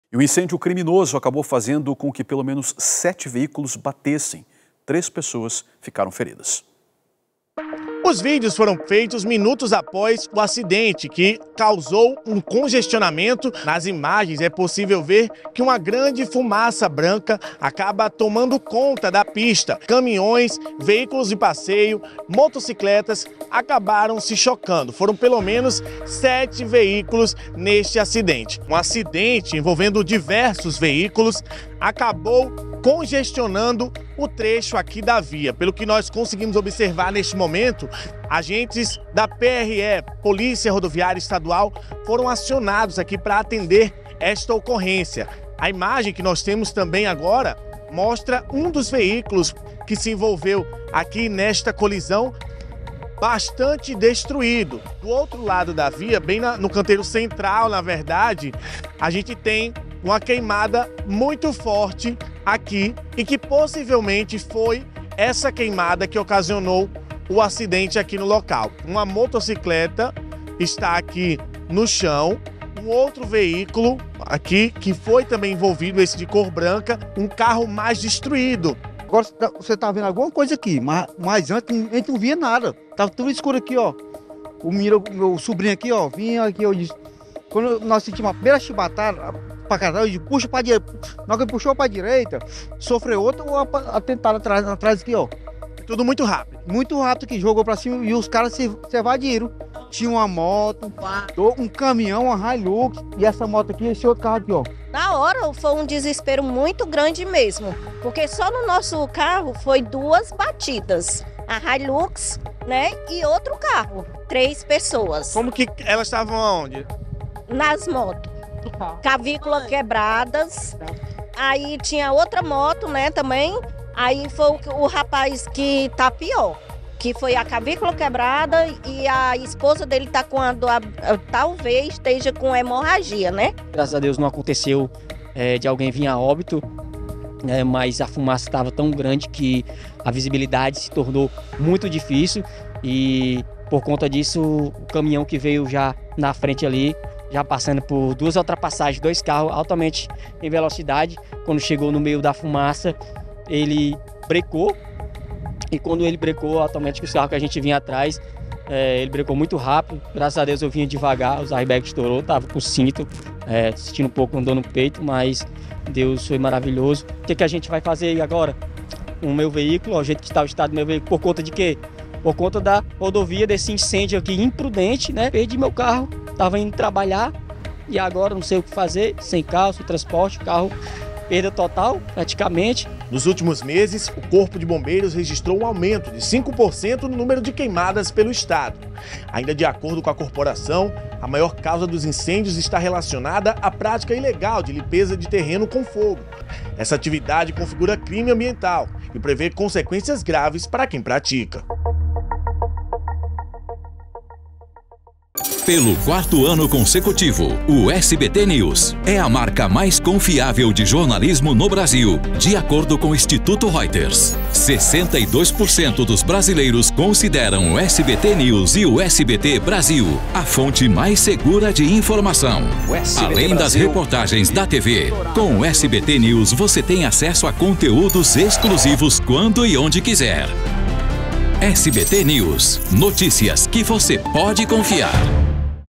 Fumaça de queimadas causa acidente com 7 carros em rodovia | SBTNewsnaTV (24/09/24)
Um incêndio criminoso acabou fazendo com que pelo menos 7 veículos colidissem. Ao todo, três pessoas ficaram feridas.